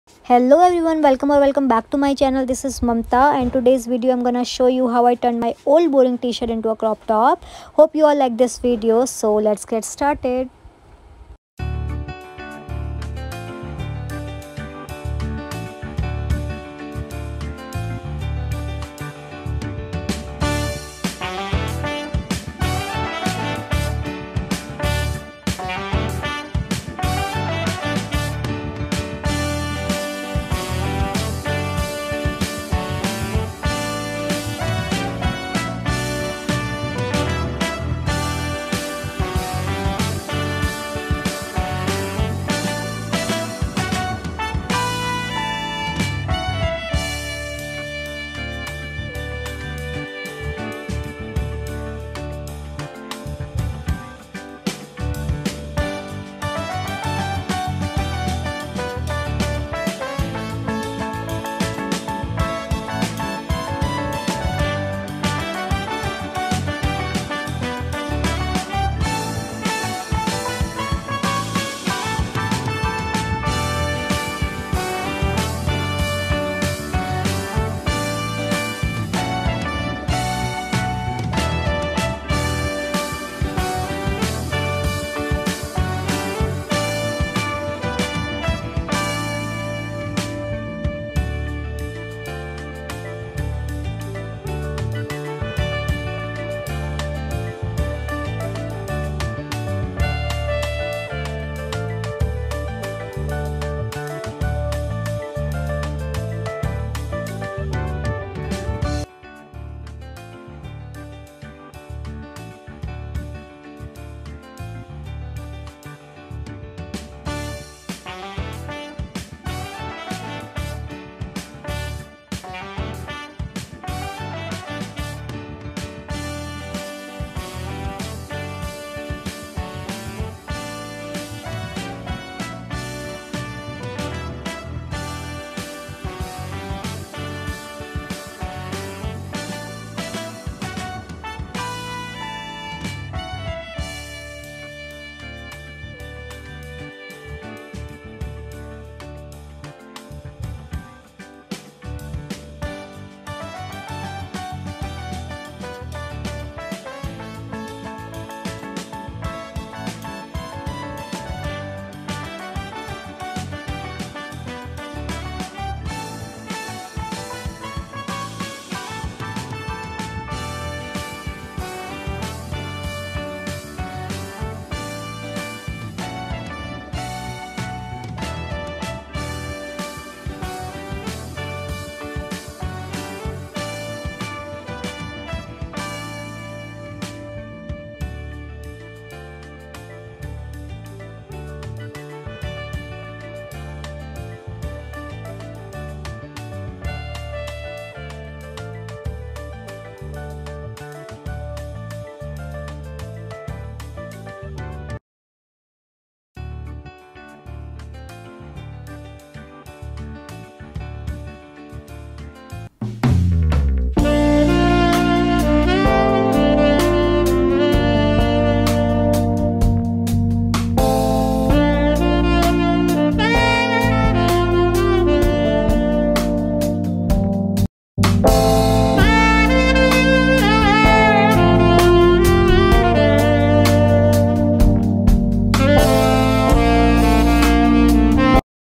DIY Idea From Old T-shirt || without any sew machine
Today’s video I am gonna show you how I turn my old boring T-shirt into a beautiful crop top.